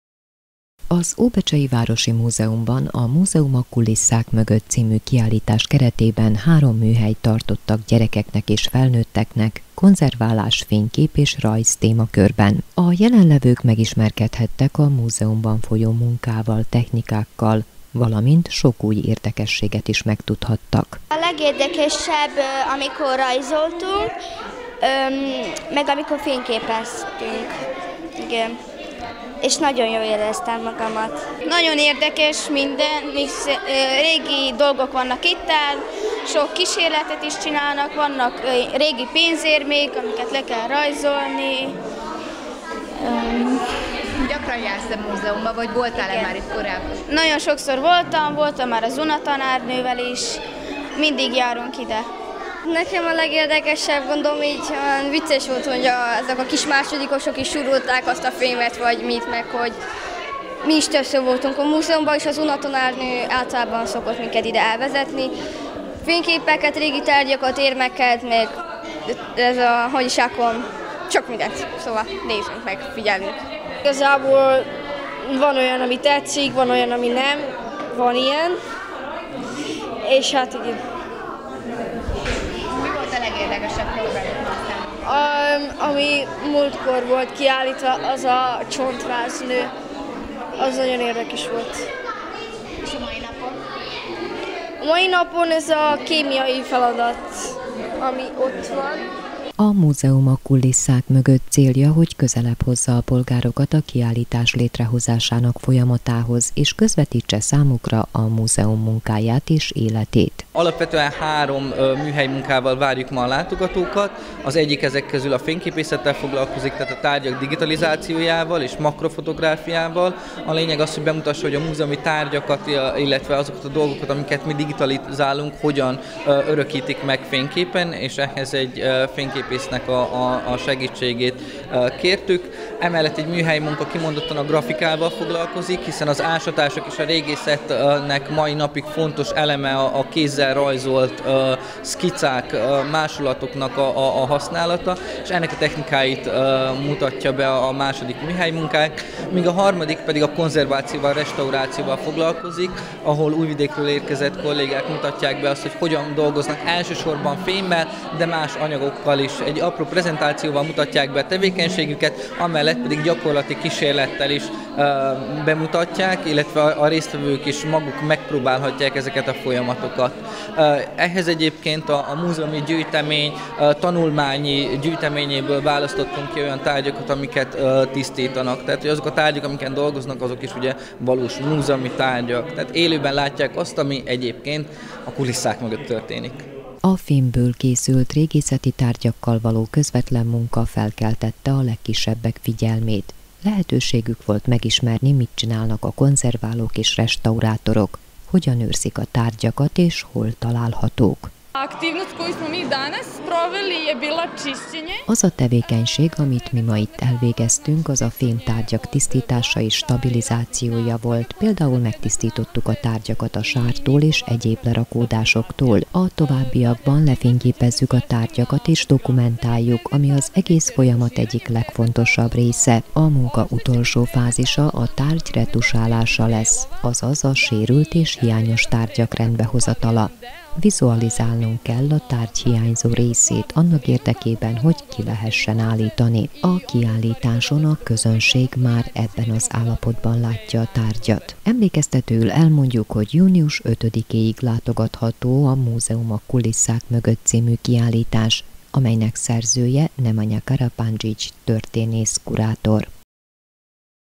TV BEČEJ: Műhelymunkák konzerválás, fotózás és rajz témakörben
TV BEČEJ: Az Óbecsei Városi Múzeumban, a Múzeum a kulisszák mögött című kiállítás keretében, három műhelyt tartottak gyerekeknek és felnőtteknek konzerválás, fénykép és rajz témakörben.